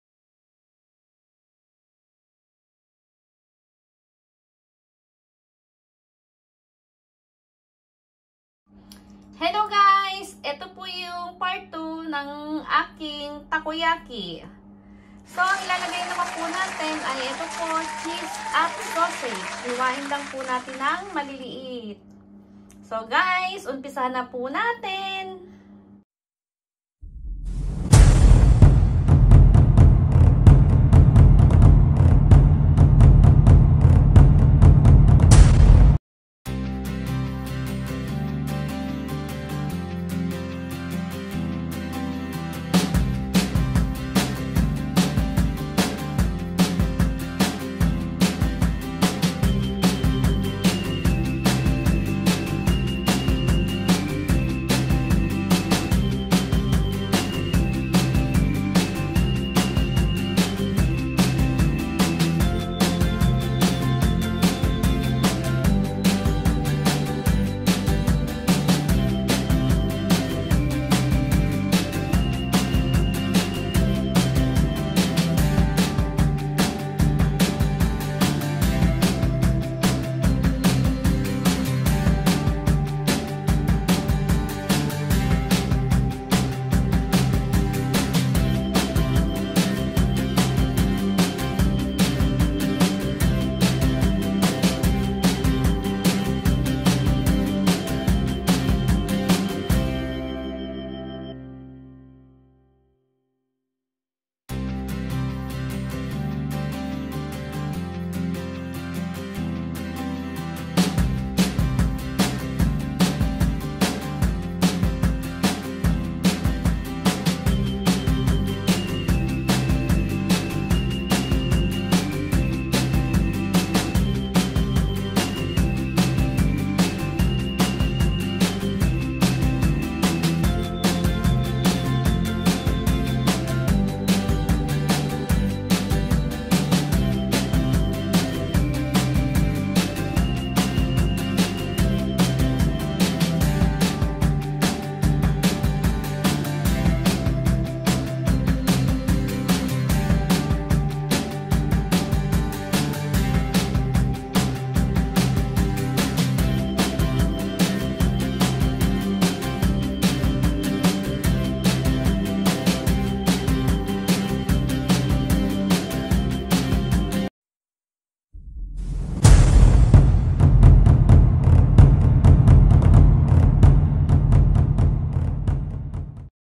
TAKOYAKI PART ll WITH SAUSAGE & CHEESE l ソーセージとチーズのたこ焼き
Takoyaki with sausage and cheese

Same ingredients i made with my previous homemade takoyaki. I just added sakura ebi or dried shrimp and much better to put the batter all at once.